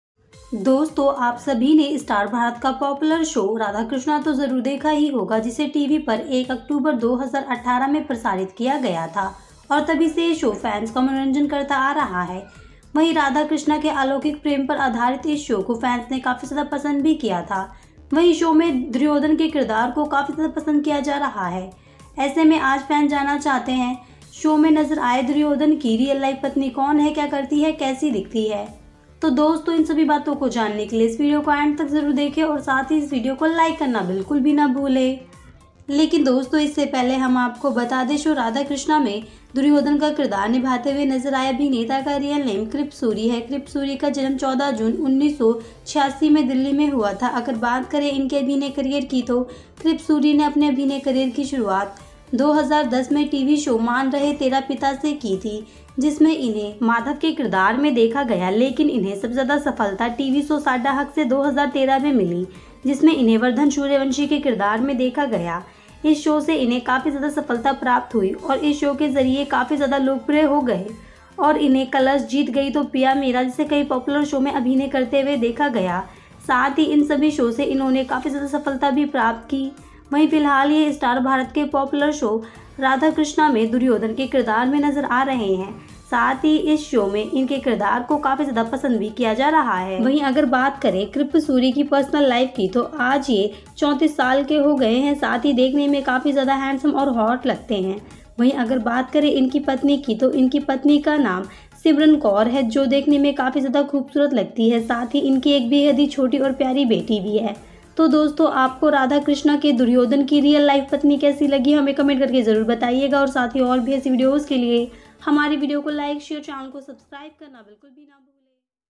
सीरियल राधा कृष्णा में नजर आए दुर्योधन की यह रियल लाइफ पत्नी ( Radhakrishna, krip suri real wife)
You all must have seen Star Plus' popular show Radha Krishna. Which was aired on 1 October 2018 on TV. In the same show, Fans liked Radha Krishna's pair. In the same show, Duryodhana's character was also given a lot of love.
In such a situation, want to know today, who is the real life wife of Duryodhana, who does what, what does it look like in the show, then friends, watch this video till the end to know things.

But friends, before we tell you, the real name of the actor who played Duryodhana in the show Radha Krishna is Kripa Suri, Krip Suri was born on 14 June 1986 in Delhi, Krupa Suri started her acting career in 2010 with the TV show Maan Rahe Tera Dad,  But Krip Suri's breakthrough TV show Sadda Haq in 2013,
 Currently, Krip Suri star is seen as Duryodhana in India's popular show Radha Krishna, Also, Fans is also liking his character of Duryodhana,

If you talk about Krip suri's personal life then
Today Kripa Suri turns 34 years old,
It also takes a lot of handsome to see,
If you talk to actor Krip Suri's wife,
Krip Suri's wife's name is Simran Kaur,
As well as his very dear daughter,

So friends, how did you like the real life wife and daughter of Duryodhana of the show Radha Krishna.  Will definitely tell us by commenting